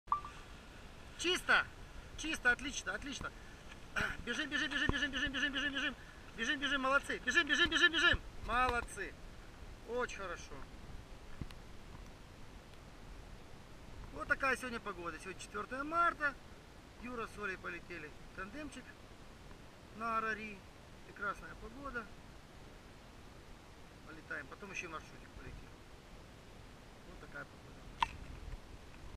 Para Israel. Ар-Ари, старт в тандеме.
Ар-Ари радовал сегодня весенними потоками и чудесной погодой. Юра катает Олю. Не зря тащил крыло из Москвы...